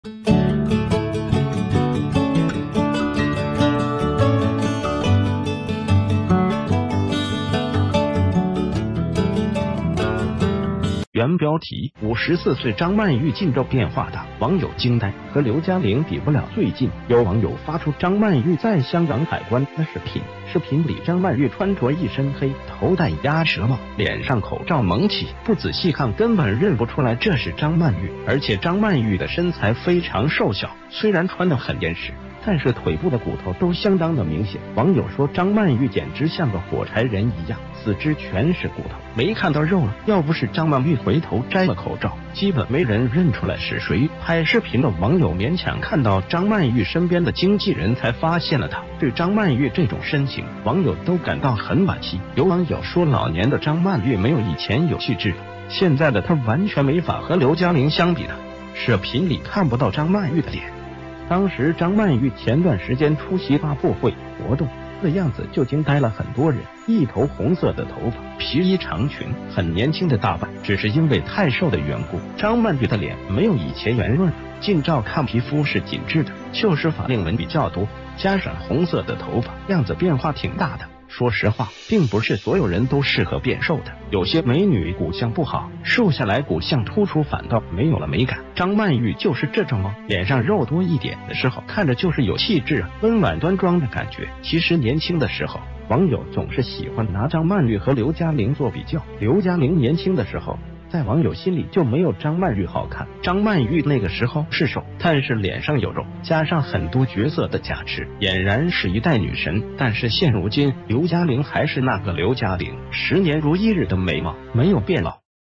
54歲張曼玉近照變化大，網友驚呆：和劉嘉玲比不了
谢谢！

      原標題：54歲張曼玉近照變化大，網友驚呆：和劉嘉玲比不了
            最近有網友發出張曼玉在香港海關的視頻，視頻里張曼玉穿著一身黑，頭戴鴨舌帽，臉上口罩蒙起，不仔細看根本認不出來這是張曼玉。

而且張曼玉的身材非常瘦小，雖然穿得很嚴實，但是腿部的骨頭都相當的明顯，網友說張曼玉簡直像個火柴人一樣，四肢全是骨頭，沒看到肉啊。

要不是張曼玉回頭摘了口罩，基本沒人認出來是誰，拍視頻的網友勉強看到張曼玉身邊的經紀人才發現了她。

對張曼玉這種身形，網友都感到很惋惜，有網友說老年的張曼玉沒有以前有氣質了，現在的她完全沒法和劉嘉玲相比啊！

視頻里看不到張曼玉的臉，當時張曼玉前段時間出席發布會活動的樣子就驚呆了很多人，一頭紅色的頭發，皮衣長裙，很年輕的打扮。

只是因為太瘦的緣故，張曼玉的臉沒有以前圓潤了，近照看皮膚是緊致的，就是法令紋比較多，加上紅色的頭發，樣子變化挺大的。

說實話，并不是所有人都適合變瘦的，有些美女骨相不好，瘦下來骨相突出反倒沒有了美感，張曼玉就是這種啊，臉上肉多一點的時候看著就是有氣質啊，溫婉端莊的感覺。

其實年輕的時候，網友總是喜歡拿張曼玉和劉嘉玲作比較，劉嘉玲年輕的時候在網友心里就沒有張曼玉好看，張曼玉那個時候是瘦，但是臉上有肉，加上很多角色的加持，儼然是一代女神。

但是現如今，劉嘉玲還是那個劉嘉玲，十年如一日的美貌，沒有變老，反而越老越有氣質。

現在的劉嘉玲看著就是大姐大的感覺，并且劉嘉玲身材凹凸有致，不瘦很有自己的氣質。

回頭看看，當初都是大美女，經過歲月的打磨 ，反而發生了兩個不一樣的變化，也是挺唏噓的。